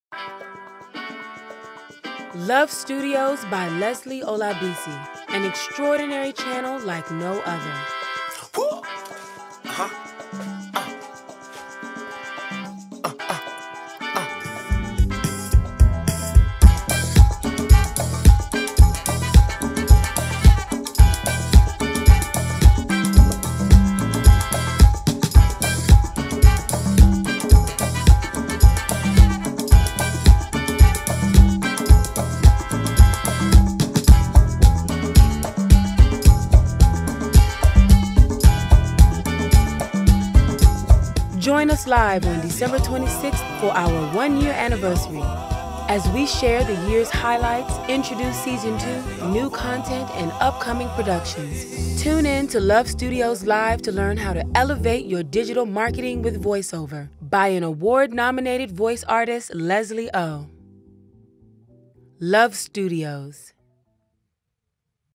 LOVStudios 2nd SEASON TRAILER - 1 Year Anniversary - December 26th
Join us on December 26th - again -  as we celebrate 1 year strong. I've learned so much and continue to bring continue and grow LOVStudios. We have new shows as well as exciting new productions in the works for 2021, including documentaries and a short film.